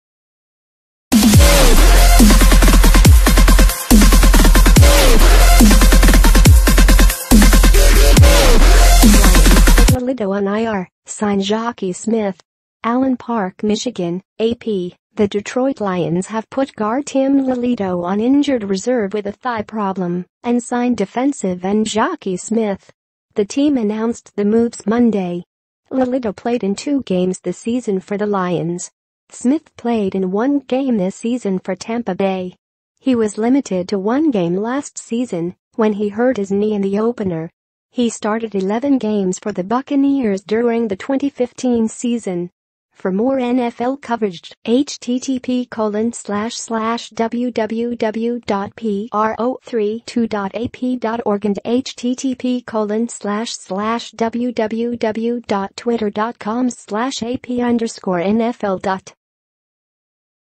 Lions put Lelito on IR, sign Jacquies Smith
ALLEN PARK, Mich. (AP) — The Detroit Lions have put guard Tim Lelito on injured reserve with a thigh problem and signed defensive end Jacquies Smith.

The team announced the moves Monday.

Lelito played in two games this season for the Lions.